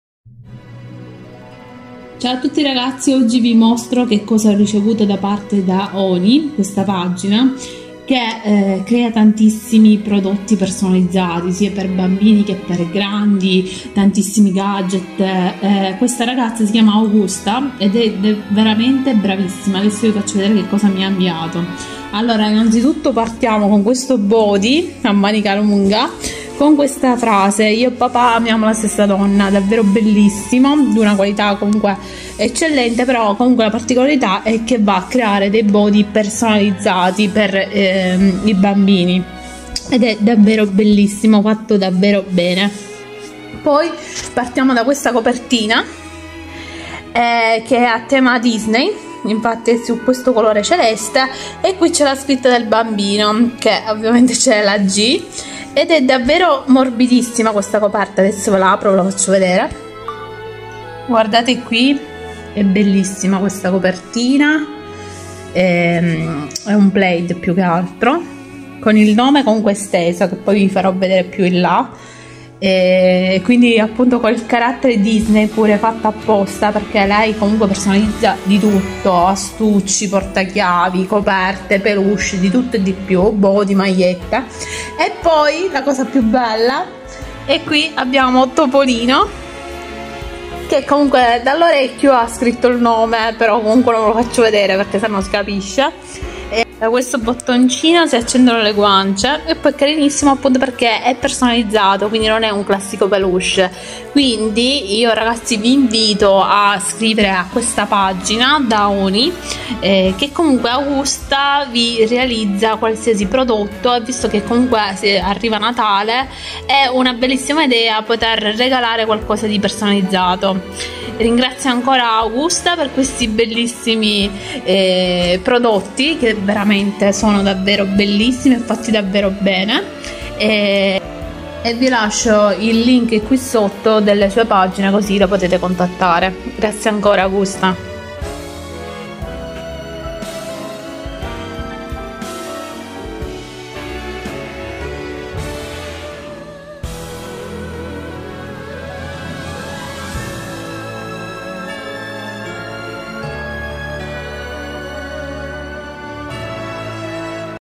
In Collaborazione con Da Onyi - prodotti personalizzati 🤩 e realizzati a mano
✨Da Onyi✨
Buon Inizio settimana Ragazzi🤗
Oggi vi parlo di @daonyiarticolipersonalizzati  ovvero di Augusta che crea tantissimi prodotti personalizzati 🤩 e devo ringraziarla per questi prodotti bellissimi 💗 sono semplicemente stupendi e poi sono anche personalizzati.

Da Onyi troverai solamente articoli fatti a mano, pezzi unici, su misura e personalizzabili.

Approfittane per fare dei regali di Natale unici 🎄
Hai tempo fino al 25 novembre per ordinare e riceverlo in tempo per Natale.

Contattate
 Vi consiglierà e progetterà assieme a voi tutto quello che volete 🤗

Grazie 💗